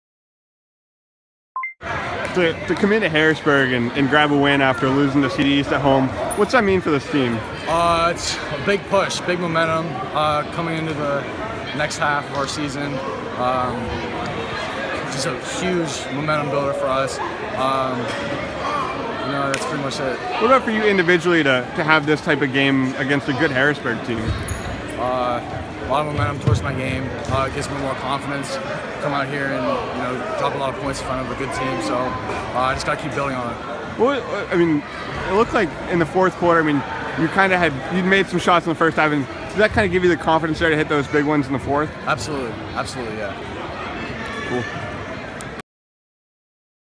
State College's Ryan McNulty on beating Harrisburg
State College's Ryan McNulty on beating Harrisburg in boys' basketball.